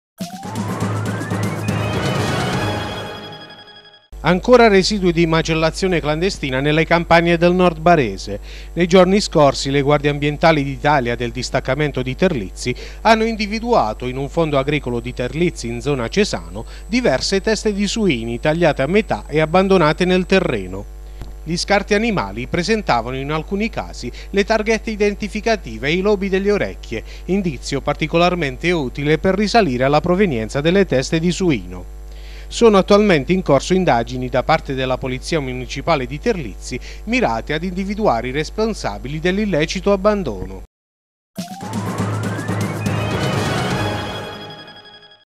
Terlizzi: teste di suino scaricate illecitamente nelle campagne
Ancora residui di macellazione clandestina nelle campagne del Nord Barese. Nei giorni scorsi le Guardie Ambientali d'Italia del distaccamento di Terlizzi hanno individuato in un fondo agricolo di Terlizzi, in zona Cesano, diverse teste di suini, tagliate a metà e abbandonate nel terreno. Gli scarti animali presentavano in alcuni casi le targhette identificative ai lobi delle orecchie, indizio particolarmente utile per risalire alla provenienza dei suini. Sono in corso indagini, da parte della Polizia Municipale, per individuare i responsabili dell'illecito abbandono.